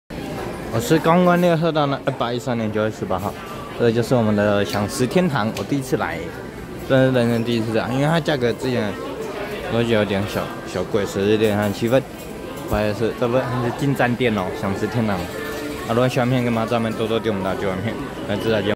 台北京站14：37分拍攝人生真的第一次來吃饗食天堂，因為之前感覺小貴不太敢來，但現在評估可以來吃吃看，今天吃完感覺京站店菜色還不賴。
112/9/18台北京站14：37分拍攝人生真的第一次來吃饗食天堂，因為之前感覺小貴不太敢來，但現在評估可以來吃吃看，今天吃完感覺京站店菜色還不賴。